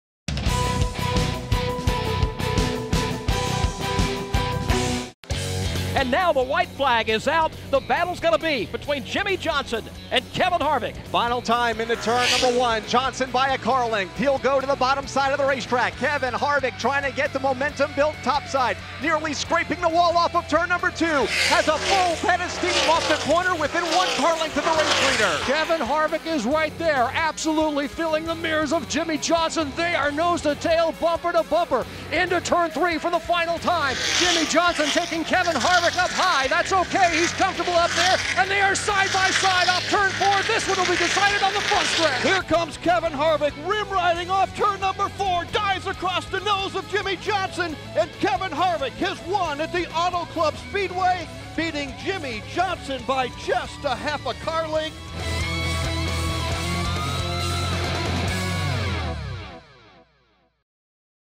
2011 Auto Club 400 - Kevin Harvick Wins
Kevin Harvick only led the final lap and that was all he needed to win the 2011 Auto Club 400 at Auto Club Speedway in Fontana, California.  Harvick tracked down Jimmie Johnson and Kyle Busch, passing Johnson in the final turn to get his lone win thus far at the California track.